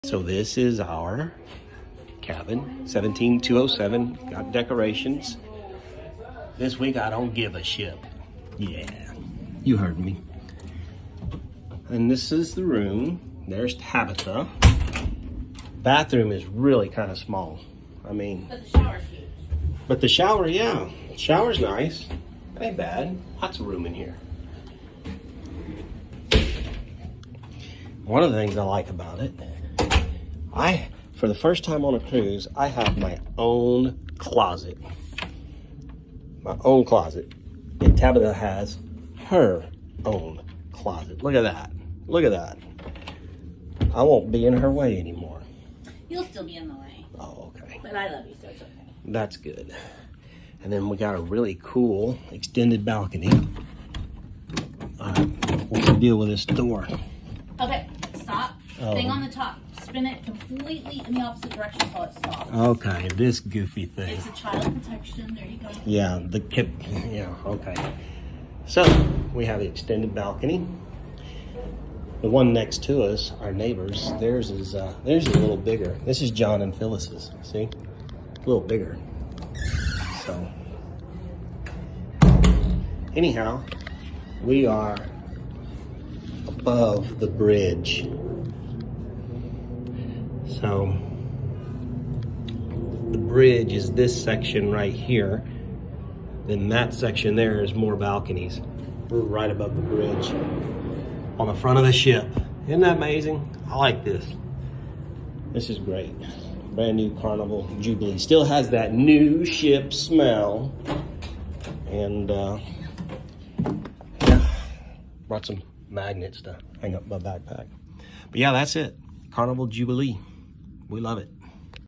Carnival Jubilee Forward Cloud9 Spa Balcony 17207
We sailed during the inaugural season of the Jubilee in February of 2024. This cabin is nice, has a lot of nice storage and features. Space between bed and wall is tight though.